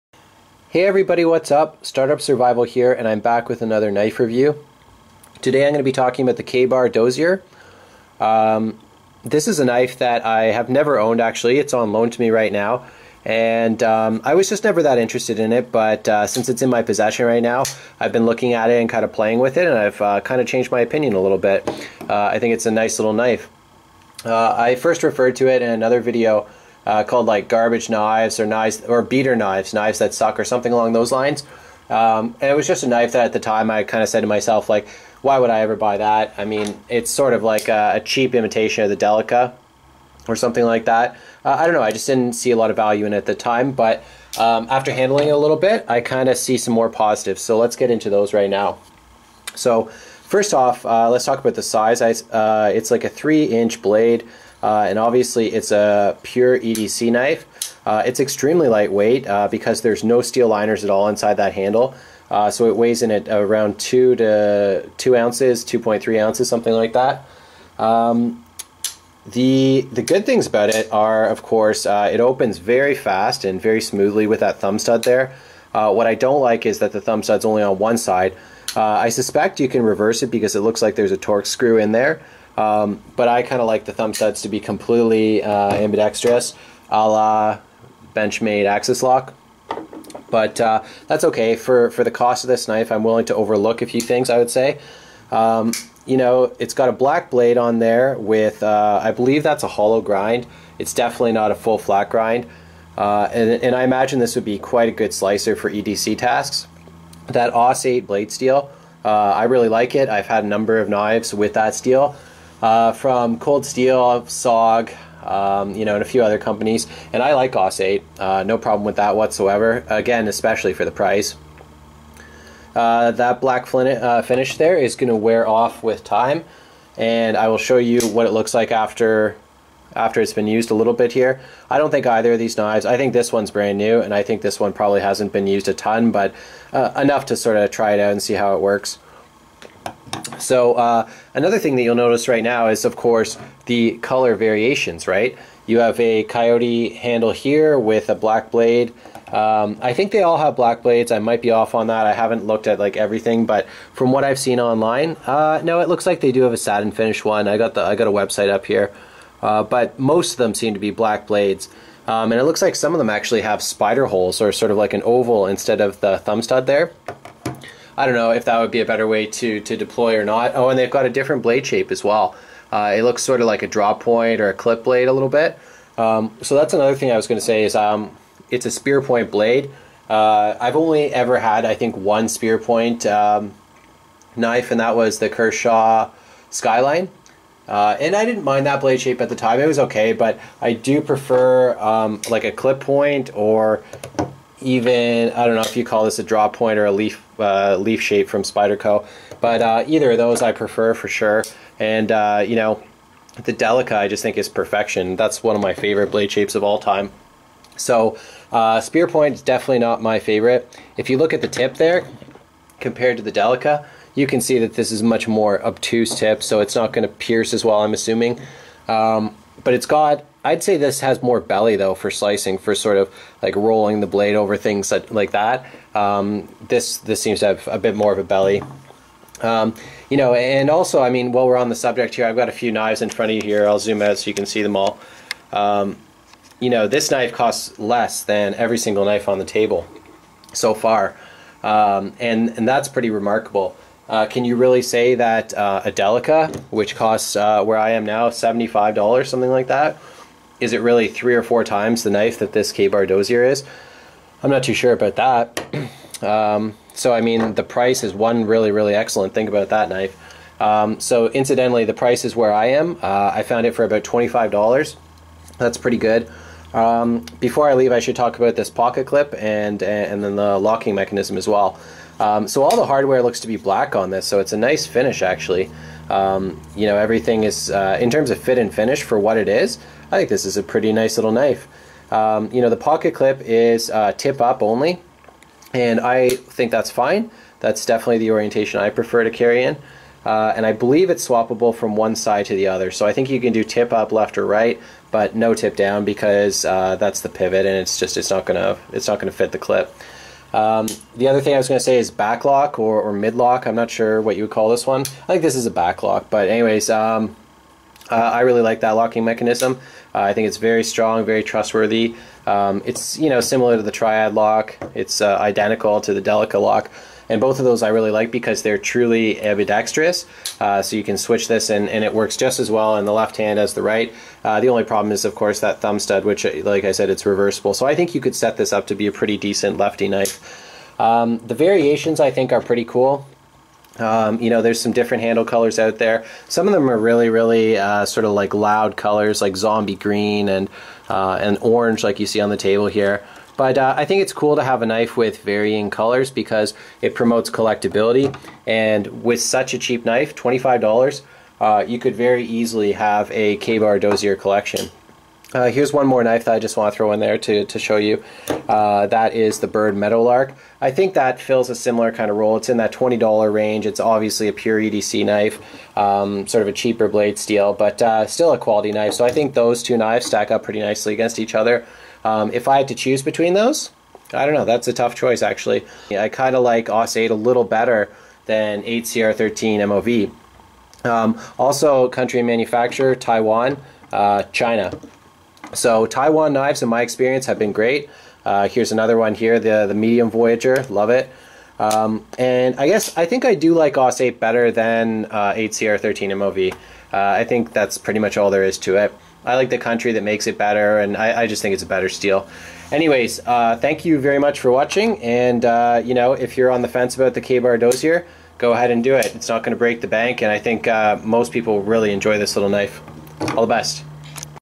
KA-Bar Dozier - Cool Budget EDC Knife
The KA-Bar Dozier is a pure EDC knife and after having a guest blade to handle, it seems like a high quality, affordable knife.  I really like Aus8 as a blade steel, and with pure FRN handles it's super light.